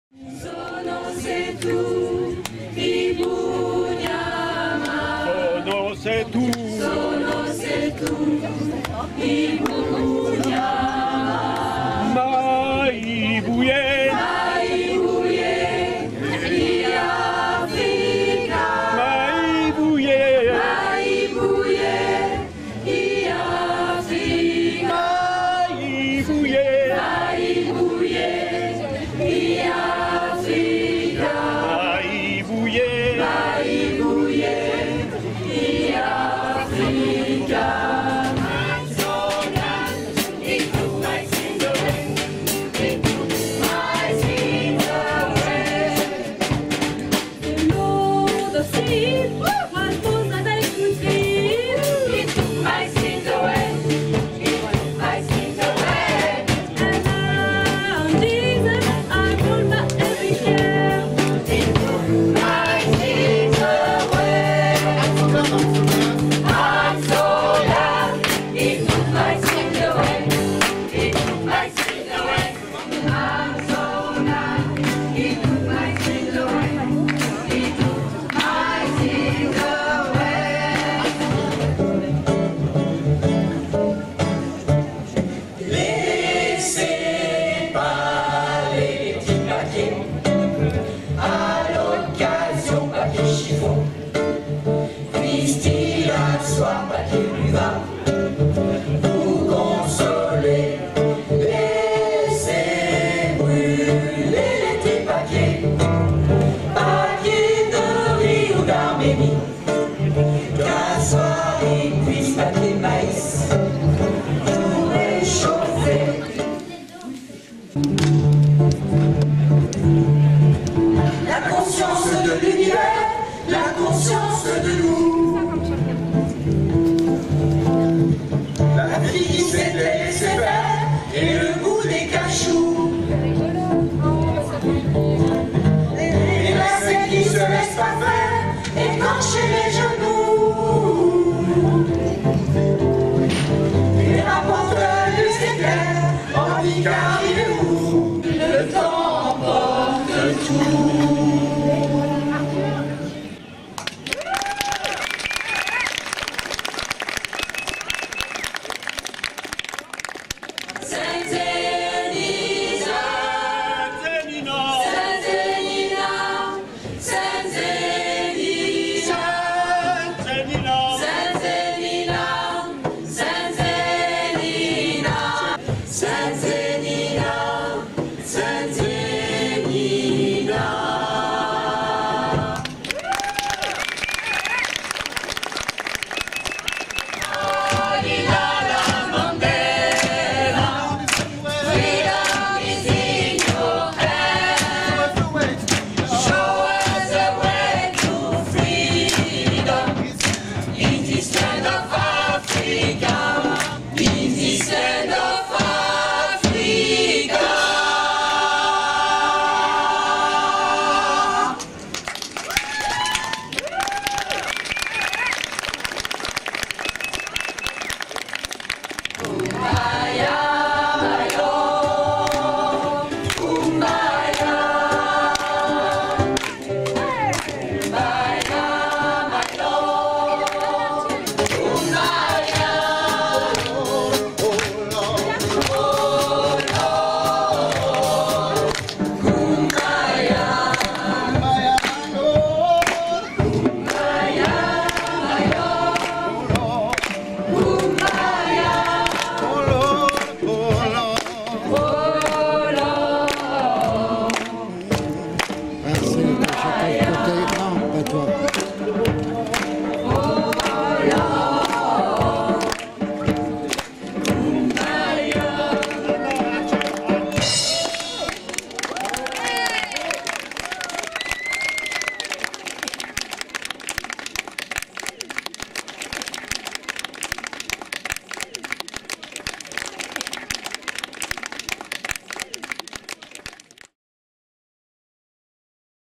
Festival En Voix la Musique: rencontre chorales
dans le cadre du festival "En Voix La Musique" du 24 au 28 Mai 2018
le 27 mai aux Cartières à Chaponost
le Festival En Voix La Musique: festival intercommunal organisé par la Mjc de Chaponost, l'Ecole de Musique de Chaponost, l'Ecole de MUsique de Vourles et l'Ecole de MUsique de Brignais